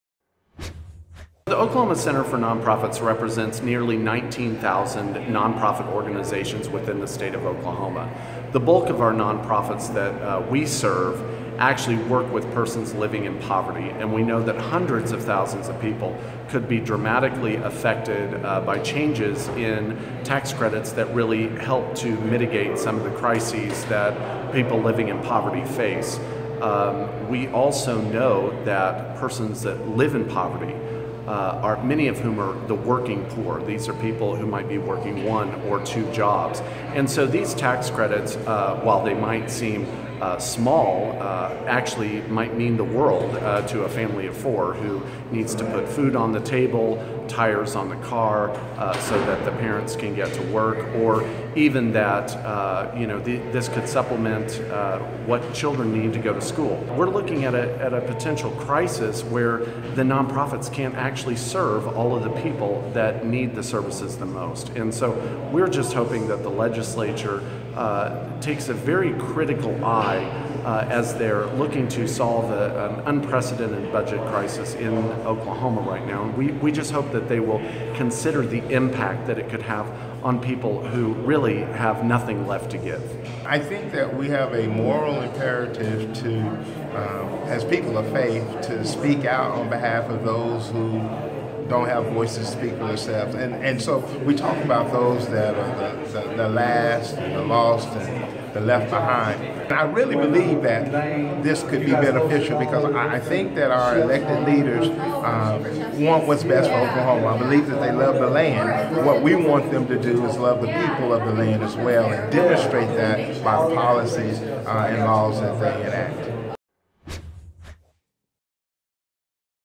Clergy and nonprofit leaders speak out against budget cuts (2016-05-11)
More than 140 religious leaders and more than two dozen nonprofit groups have signed a letter taking a stand against state budget proposals that threat to cut tax credits for working families, the disabled and senior adults.